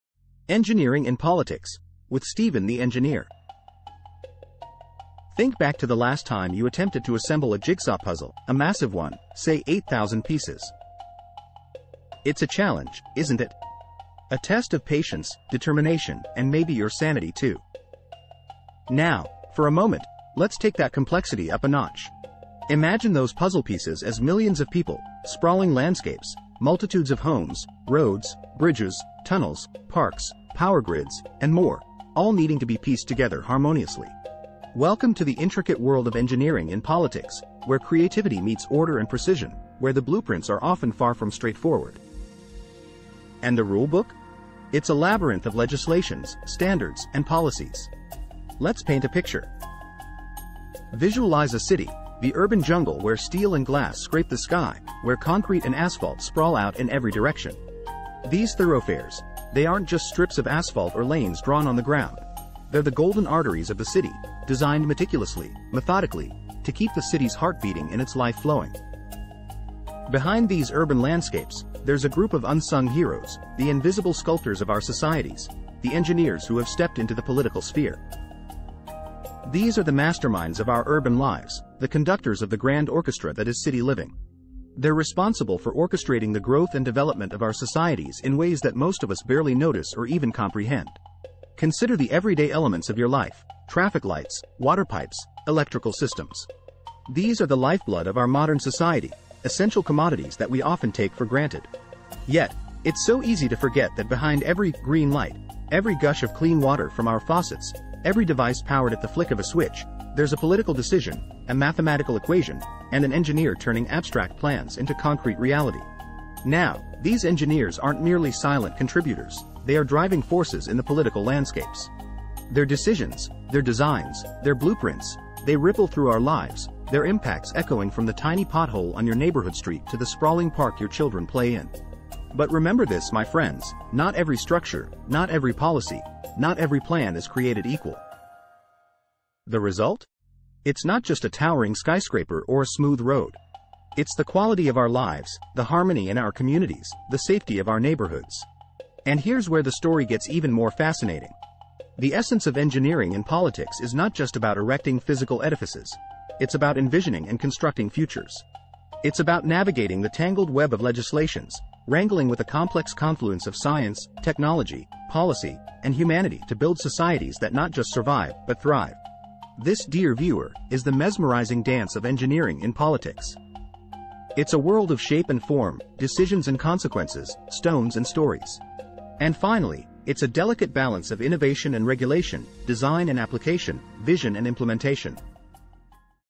Shaping Government Infrastructure: Engineering in Politics
Engineering in Politics: Shaping Government Infrastructure (Unveiled Truths) 🏗️🌐📜
Unveil the hidden truths about the role of engineering in politics, and learn how our unsung heroes shape government infrastructure in ways you'd never imagine! This video delves deep into the fascinating dance of engineering in politics, unmasking its intricate ballet of decisions, blueprints, and concrete realities.

0:00 "Engineering and Politics - A Complex Jigsaw Puzzle" 🧩💡
We kick off by drawing parallels between assembling an 8,000-piece jigsaw puzzle and piecing together a city. Intrigued yet?
0:33 "The Golden Arteries of the City" 🌆💛
We're not just talking about roads and highways here. Tune in to discover the magical, masterful orchestration of city life.
1:08 "Engineers: The Invisible Sculptors of Society" 🎩🚧
Ever thought of engineers as artists? This chapter will make you reconsider. Don't miss out on meeting the real artists behind our urban landscapes!
1:31 "Engineering Decisions: The Echoes of Life" 🚦💧⚡
From the traffic light to the water gushing from your faucets, there's a decision, an equation, and an engineer. Discover the profound impacts of these hidden equations!
2:10 "The Unseen Balance of Innovation and Regulation" ⚖️🧮🔍
Discover the mesmerizing dance of innovation and regulation, design and application, vision and implementation in engineering politics.
2:30 "Building More Than Just Buildings" 🏢🌳👨‍👩‍👧‍👦
Unearth how engineers in politics are constructing futures, shaping societies that don't just survive, but thrive.

Peel back the layers of the urban world around you and gain a whole new perspective on the unseen forces that shape our lives every day. Ready to take the plunge? Let's dive in! 🏊‍♀️🚀🌍